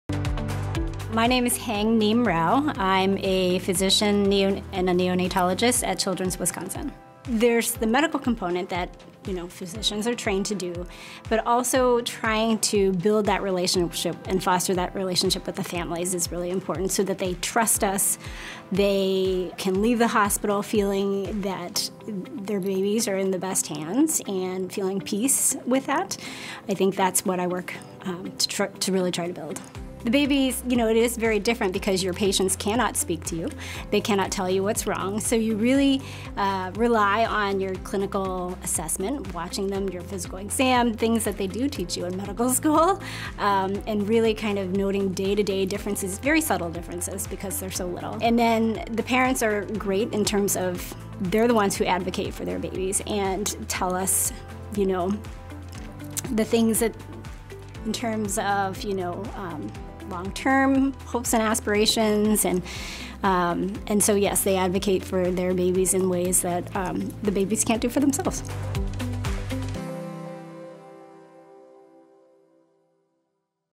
Tuyet-Hang Nghiem-Rao, MD, from Children's Wisconsin
Dr. Nghiem-Rao is a neonatologist at Children's Wisconsin. To learn more about newborn and fetal care at Children's Wisconsin,